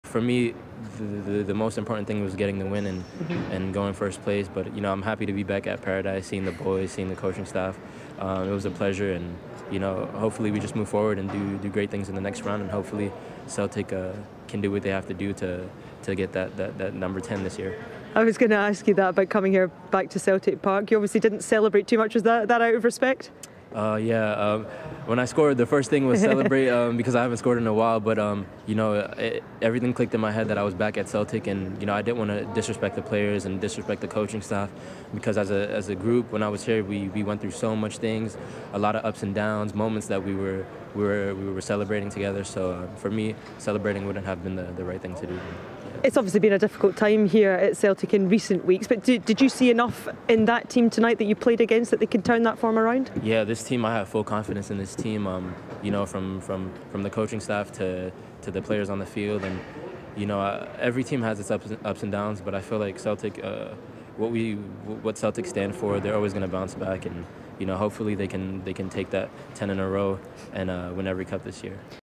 "What Celtic stand for, they are always going to bounce back." Timothy Weah is forever a Bhoy
Once a Bhoy, always a Bhoy! Timothy Weah reflects on a return to Paradise.